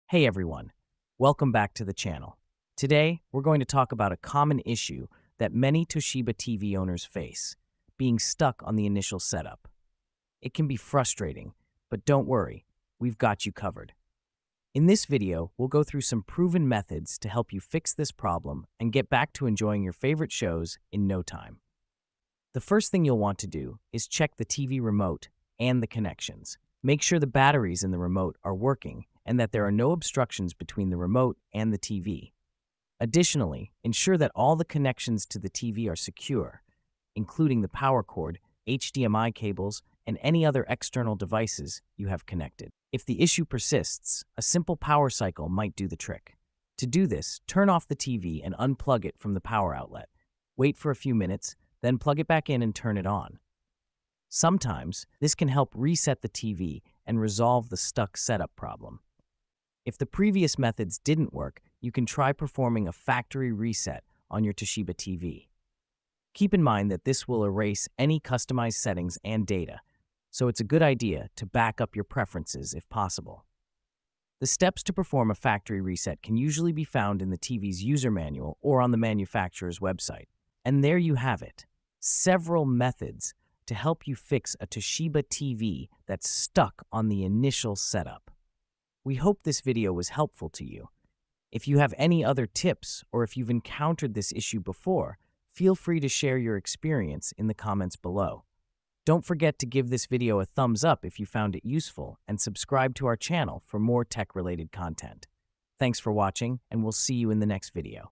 How to Fix Toshiba TV Stuck On Initial Setup
Is your Toshiba TV stuck on the initial setup screen? Watch our step-by-step guide to learn how to quickly troubleshoot and resolve this common issue. Don't let a frozen setup screen stop you from enjoying your Toshiba TV. Try these easy methods now!